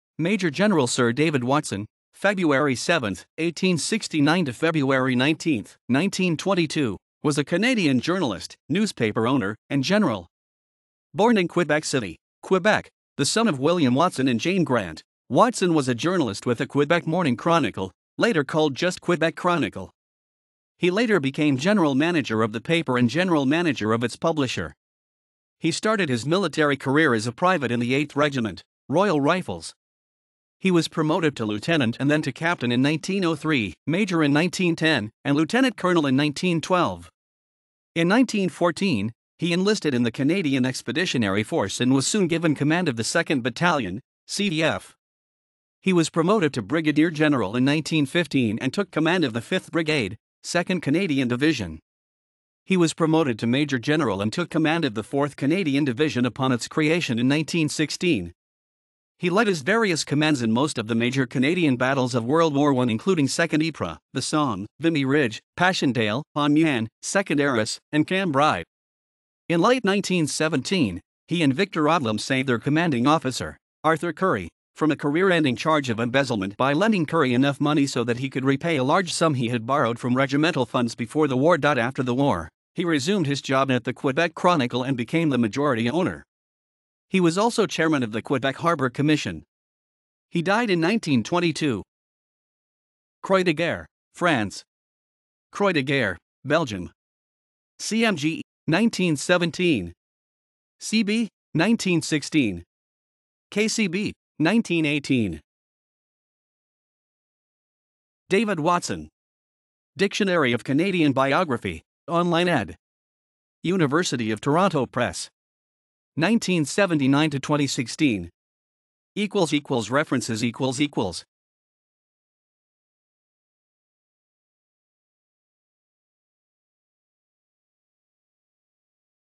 David Watson (General)
Major-General Sir David Watson,  (7 February 1869 – 19 February 1922) was a Canadian journalist, newspaper owner, and General.
Born in Quebec City, Quebec, the son of William Watson and Jane Grant, Watson was a journalist with the Quebec Morning Chronicle (later called just Quebec Chronicle). He later became general manager of the paper and general manager of its publisher.
He started his military career as a private in the 8th Regiment, Royal Rifles. He was promoted to lieutenant and then to captain in 1903, major in 1910, and lieutenant-colonel in 1912. In 1914, he enlisted in the Canadian Expeditionary Force and was soon given command of the 2nd Battalion, CEF. He was promoted to Brigadier-General in 1915 and took command of the 5th Brigade, 2nd Canadian Division. He was promoted to Major-General and took command of the 4th Canadian Division upon its creation in 1916. He led his various commands in most of the major Canadian battles of World War I including Second Ypres, the Somme, Vimy Ridge, Passchendaele, Amiens, Second Arras, and Cambrai.
In late 1917, he and Victor Odlum saved their commanding officer, Arthur Currie, from a career-ending charge of embezzlement by lending Currie enough money so that he could repay a large sum he had borrowed from regimental funds before the war.After the war, he resumed his job at the Quebec Chronicle and became the majority owner. He was also chairman of the Quebec Harbour Commission. He died in 1922.